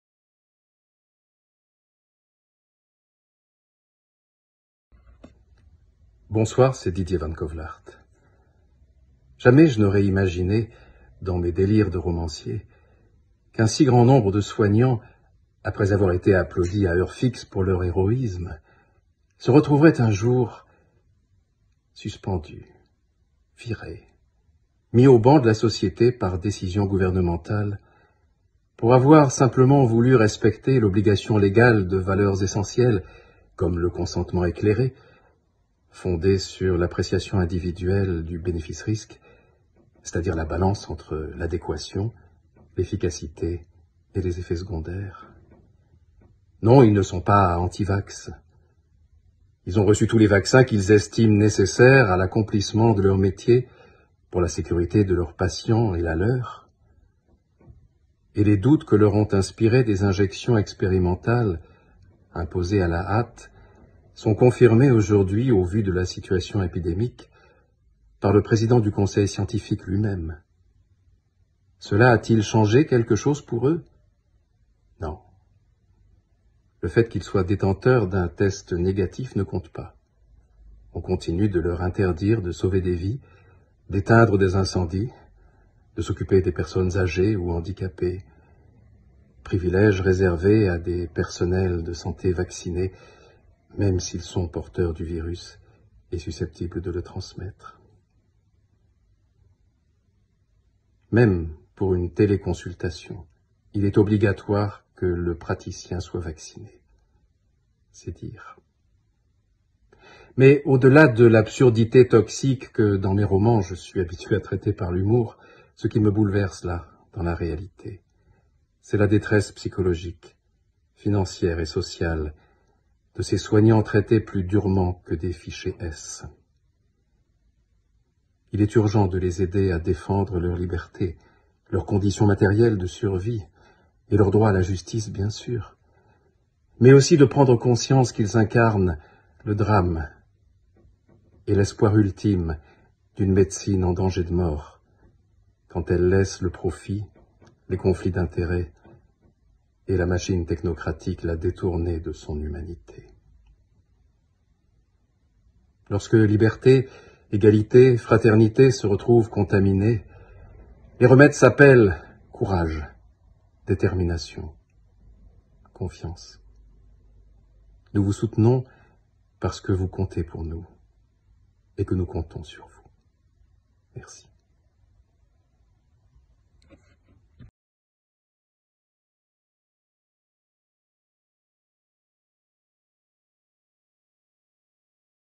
Didier VAN CAUWELAERT, Soutien aux soignants!
Des soignants traités plus durement que des "Fiché S"